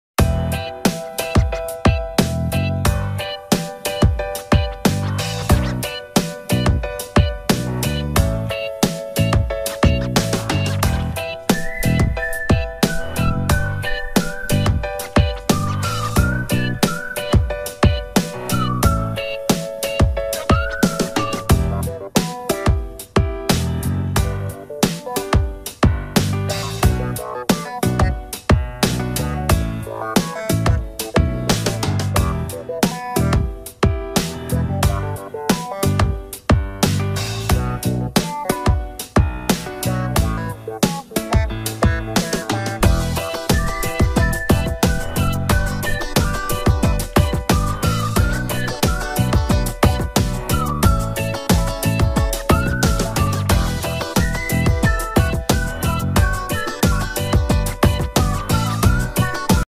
WhatsApp New Update (September 2020)
Hello viewers,I will show you in this video Whatsapp Very important update.This update very helpful for every whatsapp users,